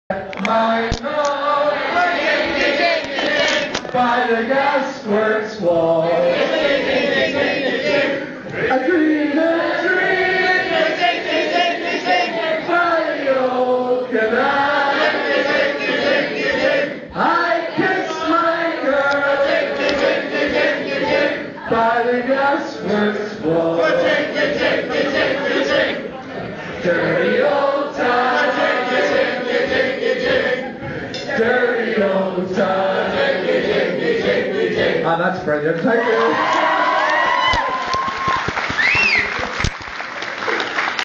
Best man sing song.MP4
Geri and James's Wedding 29/12/09 Best man made us sing! :D